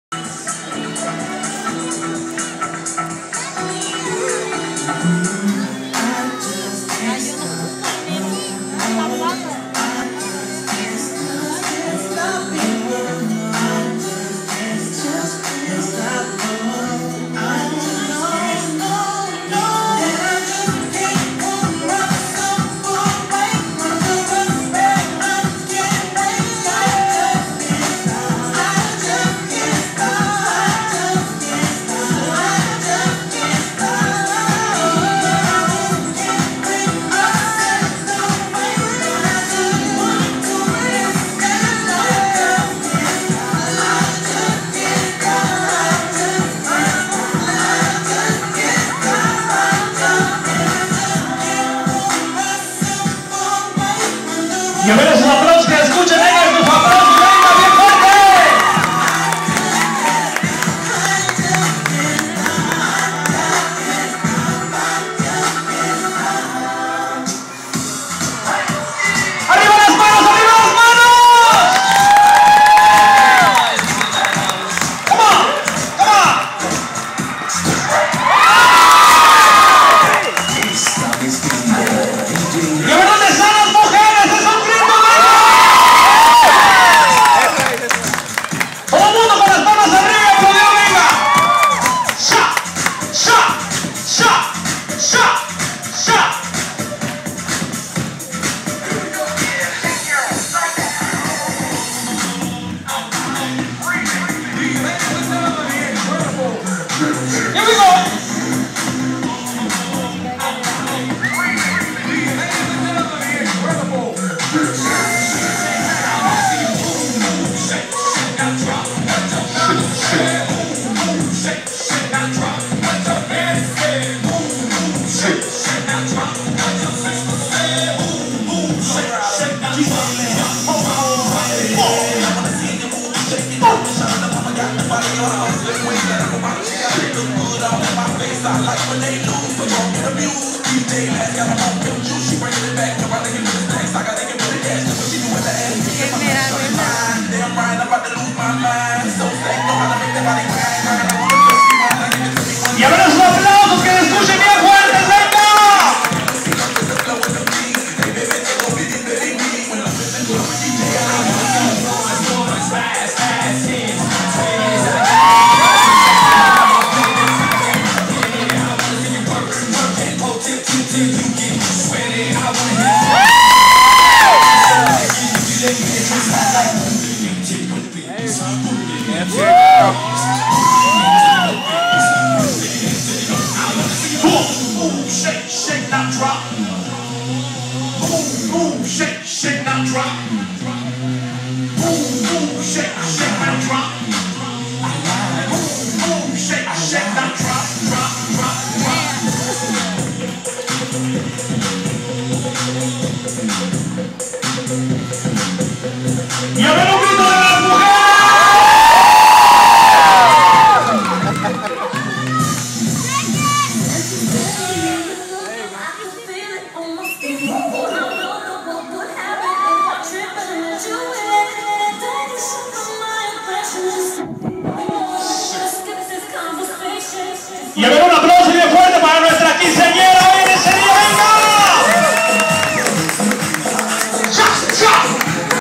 Banessa 15,s suprise dance
Banessa dancing on her 15,s party
In AR